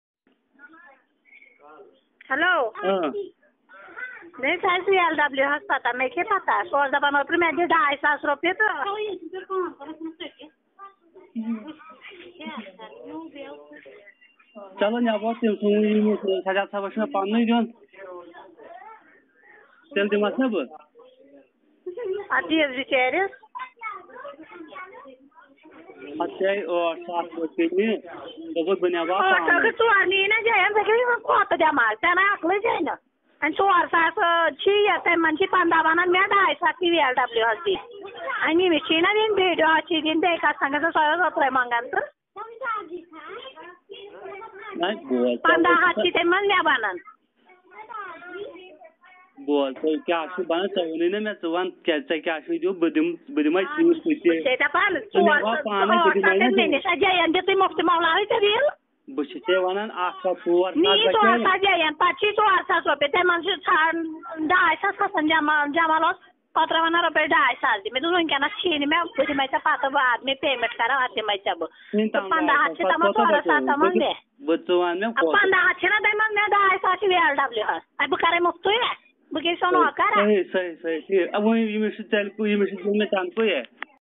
BJP Sarpanch Yasmeena Begum of Qadrina-A in Marwah block of Kishtwar accepting to corrupt practices
Audio conversation of BJP Sarpanch Yasmeena Begum of Qadrina-A in Marwah block of Kishtwar District with local Rustum Ali accepting to corrupt practices